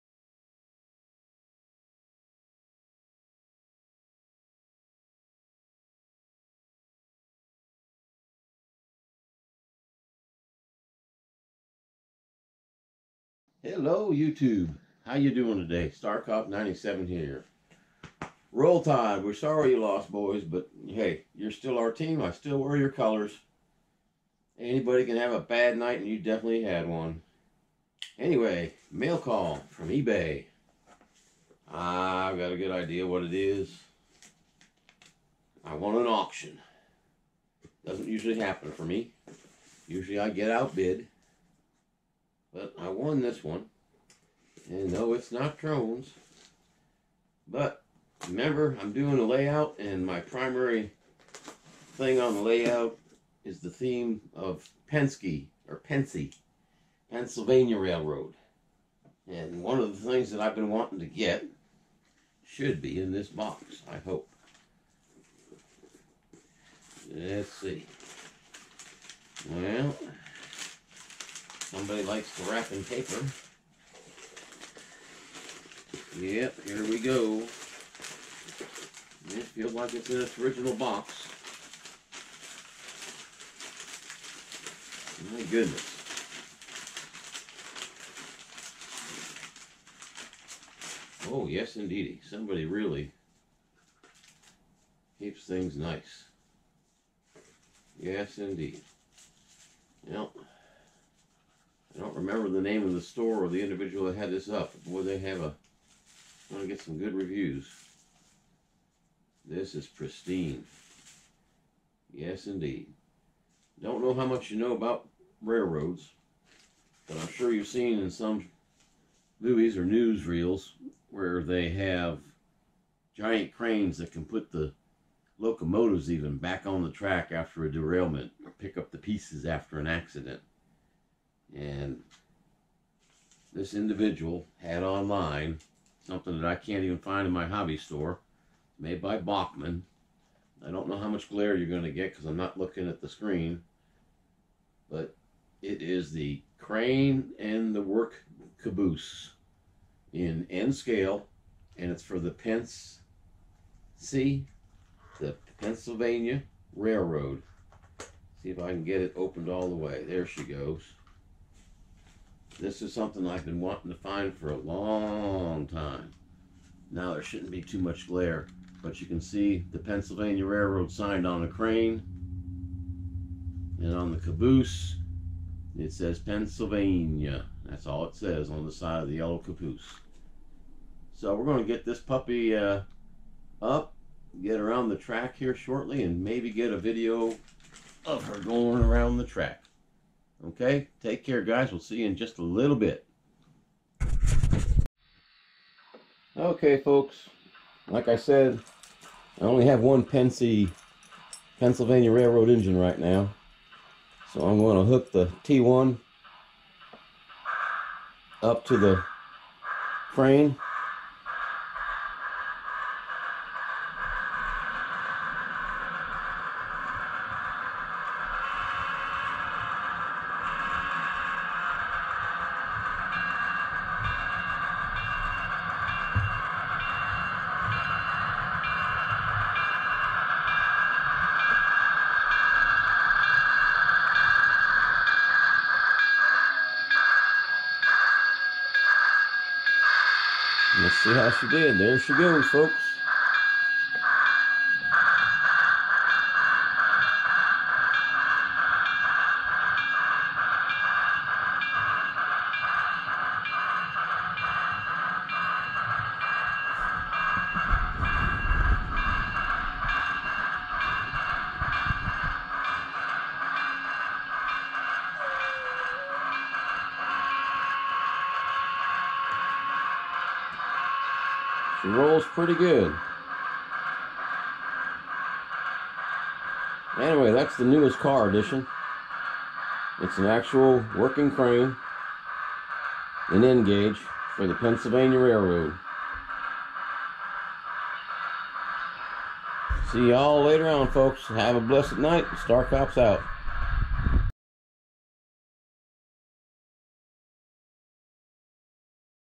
Mail Call PRR Crane & Caboose
Box Opening PRR Crane and Caboose with first use on the layout!